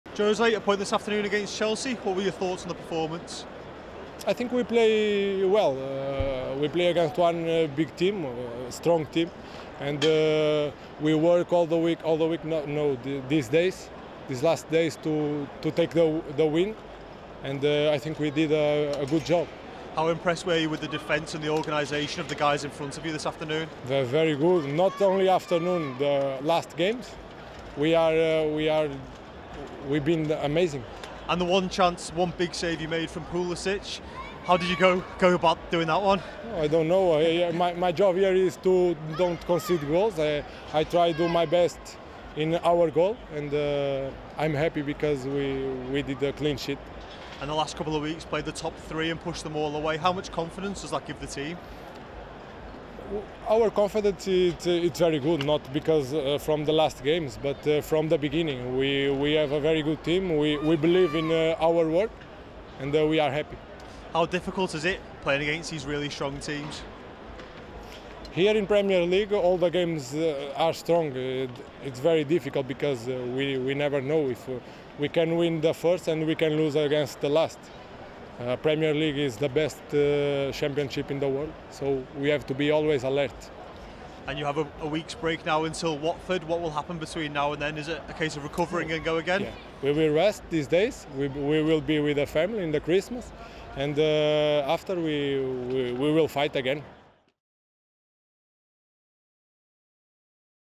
Sa on his clean sheet v Chelsea
Jose Sa spoke with Wolves TV's Mark Ridgway following the 0-0 draw with Chelsea at Molineux.